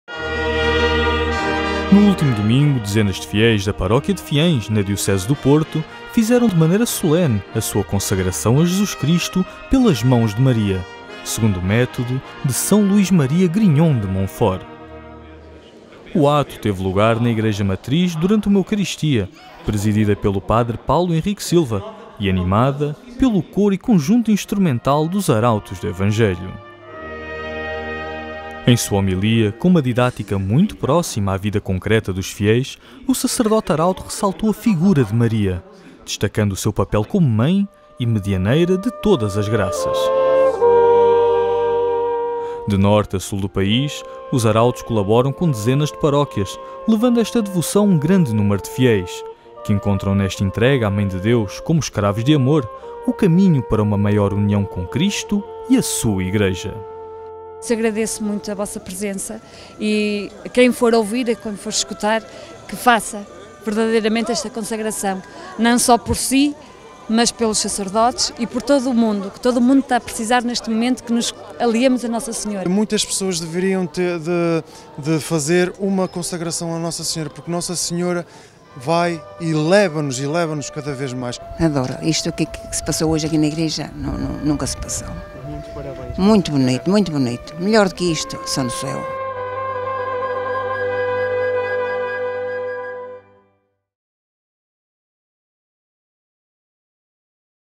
Consagração a Jesus pelas mãos de Maria
Dezenas de fiéis da paróquia de Fiães, na Diocese do Porto, fizeram de maneira solene a sua consagração a Jesus Cristo pelas mãos de Maria, segundo o método de São Luís Maria Grignion de Montfort. Assista aqui ao resumo desta bela cerimónia animada pelo coro e banda da instituição.